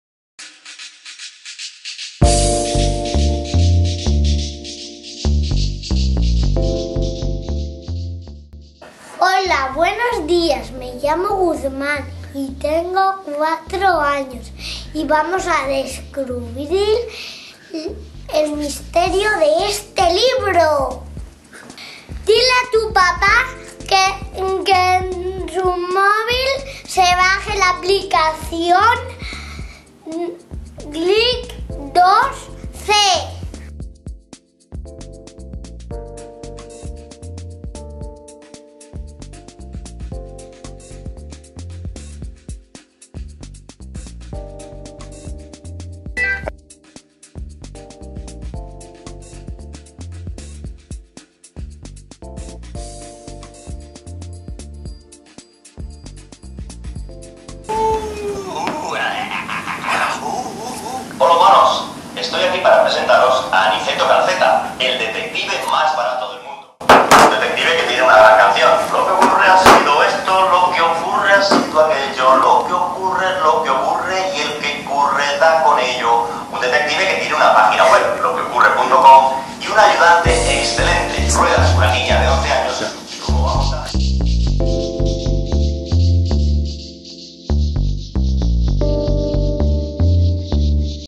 Guzmán nos presenta los misterios de Anizeto Calzeta con CLIC2C
El último libro de Guillermo Fesser,  El misterio de los fantasmas solitarios con Anizeto Calzeta como protagonista. Usa CLIC2C sobre el libro para resolver los misterios... uuuuuuhhh!!!